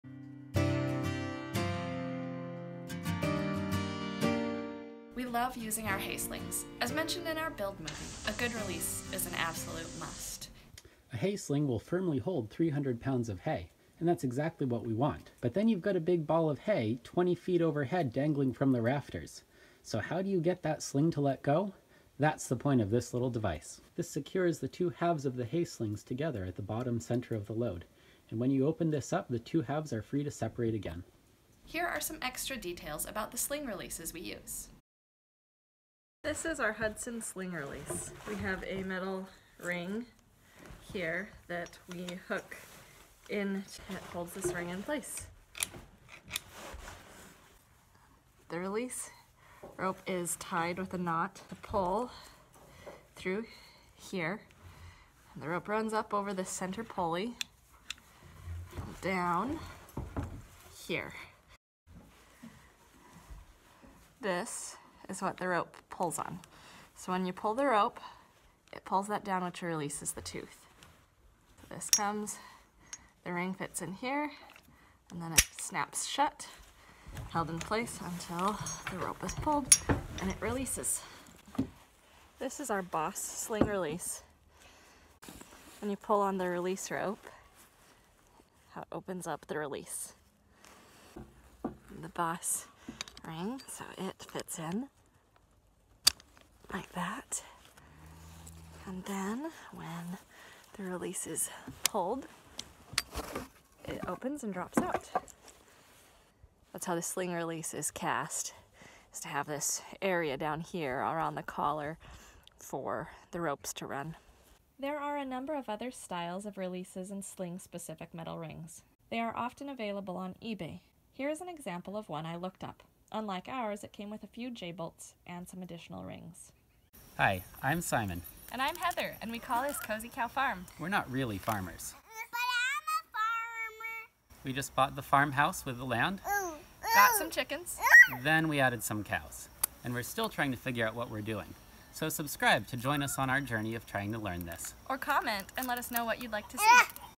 Hay Slings Part 2: Sling Releases
We love using our hay slings. As mentioned in our build movie, a good release is an absolute must if you want to use a hay sling to put up hay. Here are some extra details about the sling releases we use.